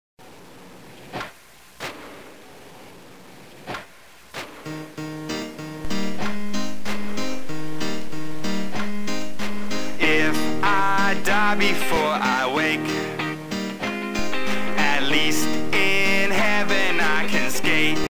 ollie off 3-foot ledge
please don't put any bad comments. my friend and i skate off a 3-foot ledge.